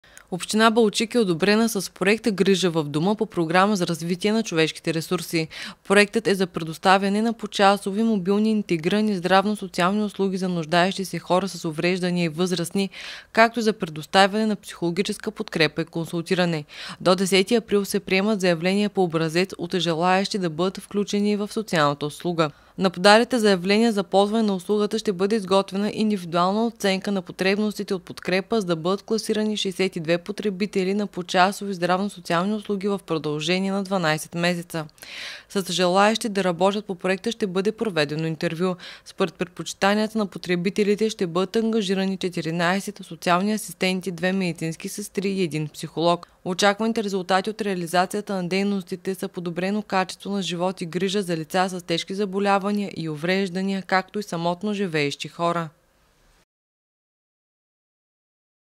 Над 60 нуждаещи се в община Балчик получават „Грижа в дома“
Община Балик е одобрена с проект „Грижа в дома“ по Програма за развитие на човешките ресурси. Проектът е за предоставяне на почасови мобилни интегрирани здравно-социални услуги за нуждаещи се хора с увреждания и възрастни, както и за предоставяне на психологическа подкрепа и консултиране.

До 10 април се приемат заявления по образец от желаещи да бъдат включени в социалната услуга. Кандидатите да я предоставят като социални асистенти, медицински специалист и психолог подават документи в същия срок.

На подалите заявления за ползване на услугата ще бъде изготвена индивидуална оценка на потребностите от подкрепа, за да бъдат класирани 62 потребители на почасови здравно-социални услуги в продължение на 12 месеца.

С желаещите да работят по проекта ще бъде проведено интервю. Според предпочитанията на потребителите ще бъдат ангажирани 14 социални асистенти, две медицински сестри и един психолог. Очакваните резултати от реализацията на дейностите са подобрено качество за живот и грижа за лица с тежки заболявания и увреждания и самотно живеещи хора; намален риск от зависимост от институционален тип грижи.